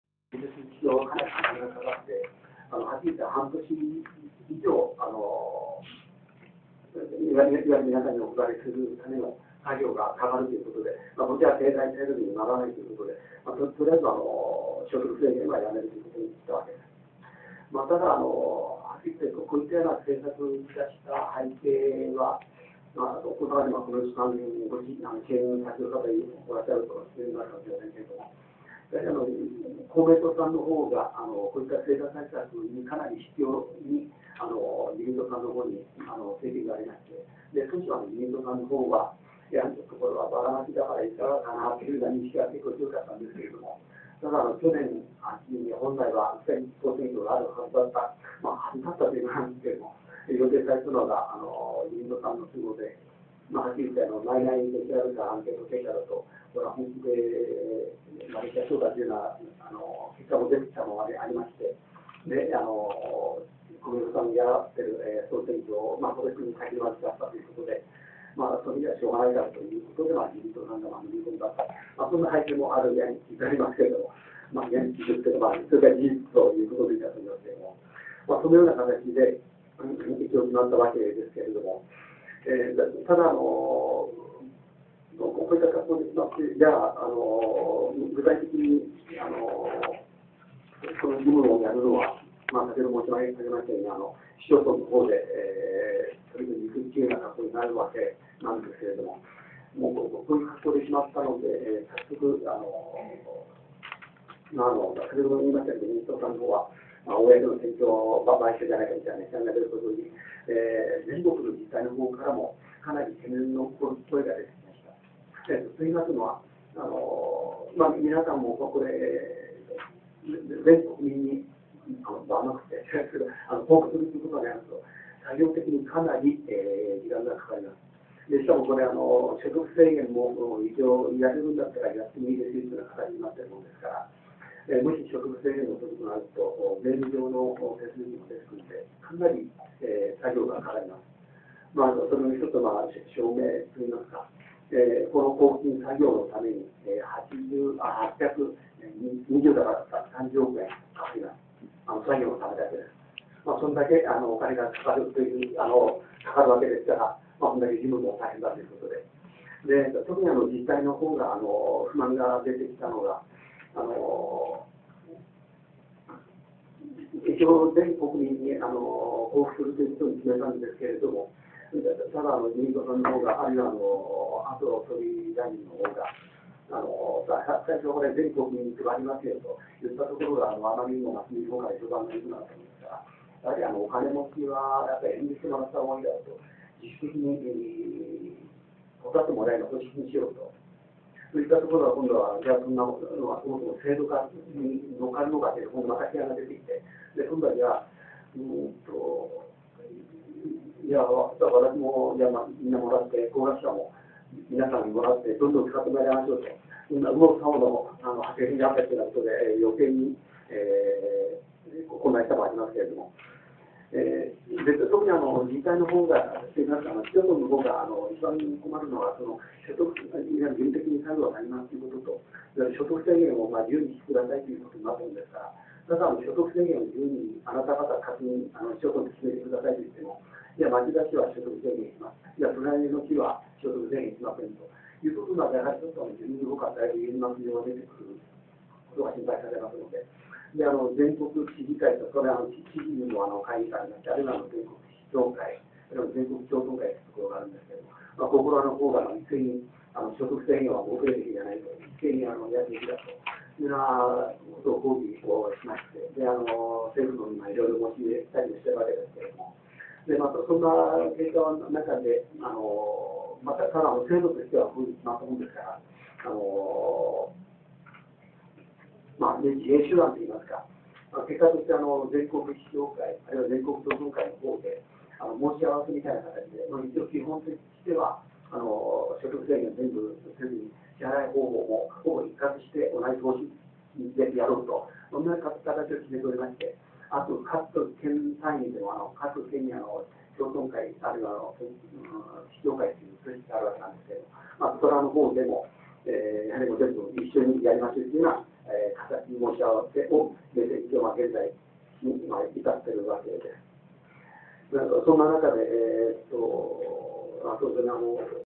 第12回セミナーvol.2 定額交付金の支給制限
吉田つとむ後援会の第12回セミナーは、vol.2 定額交付金の支給制限の問題から始まる。麻生首相が言い出した所得制限の発想。作業事務費　830億円　記:吉田つとむ